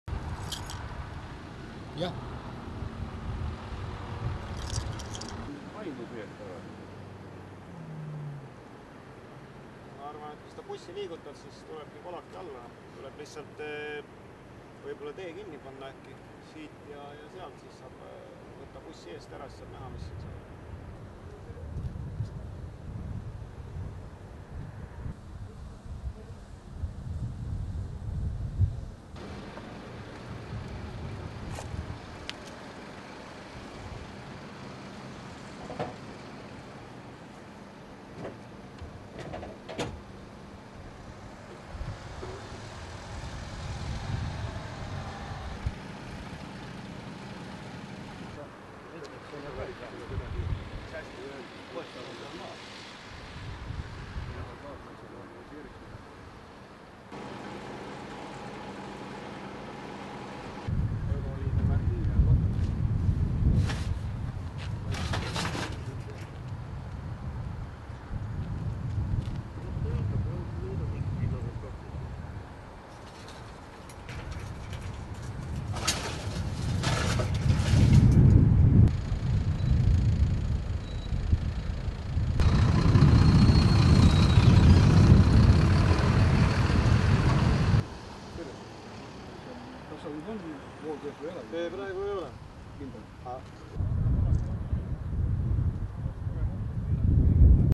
Kaubik rammis väikebussi teelt välja
Rakveres said Karja ja Võidu tänava ristmikul kokku kaks sõidukit. Volkswageni kaubiku juht ei pannud tähele anna teed liiklusmärki ja  andis Iveco väikebussile müksu, mille peale rammis see mitmeid teele jäänud liikluskorraldusvahendeid. Iveco hoo peatas suur reklaamtahvel.